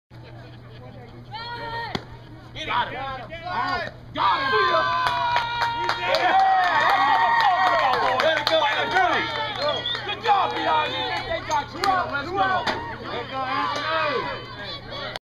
Andrew throws out runner trying to steal third, 3/15/15
11U tournament, USSSA, Walterboro, SC